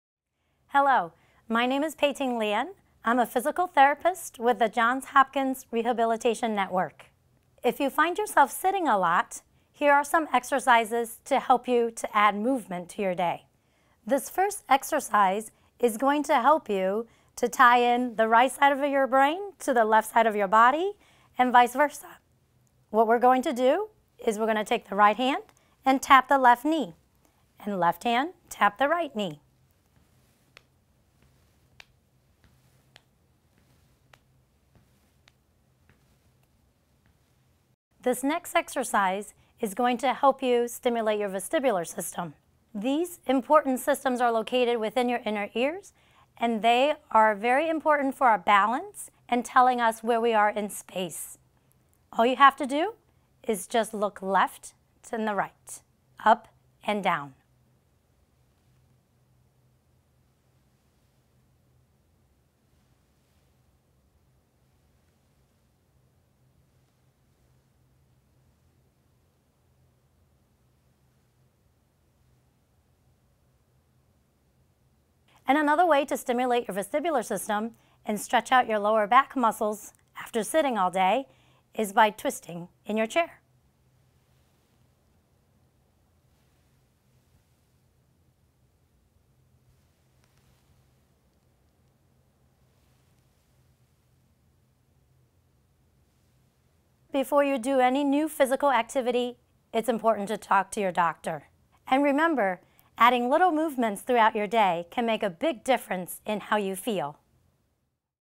Simple Exercises to Move More Throughout the Day with Physical Therapist Peiting Lien
It’s important to incorporate a little movement throughout the day, even at an office job. Simple exercises can have a big impact on how you feel physically and mentally. Physical therapist Peiting Lien from the Johns Hopkins Rehabilitation Network demonstrates and explains three easy exercises that can be done in a chair.